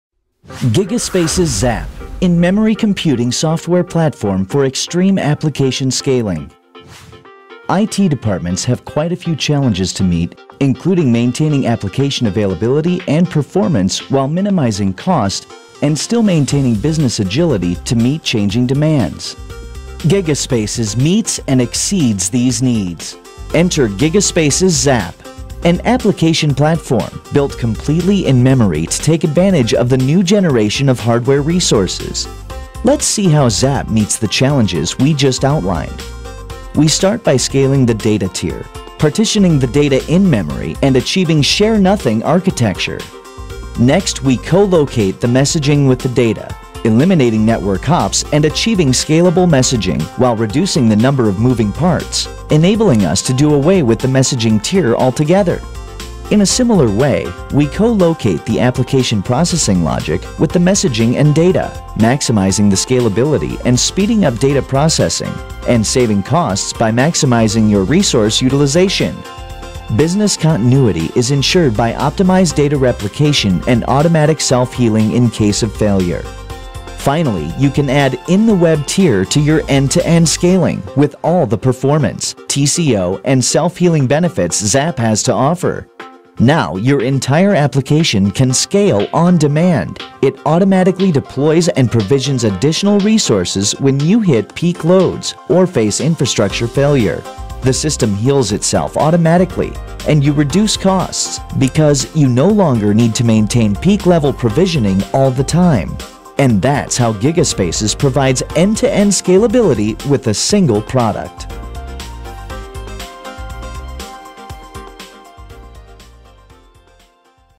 XAP In-Memory Computing Software Platform
XAP is an in-memory computing software platform that processes all of your data and events in real time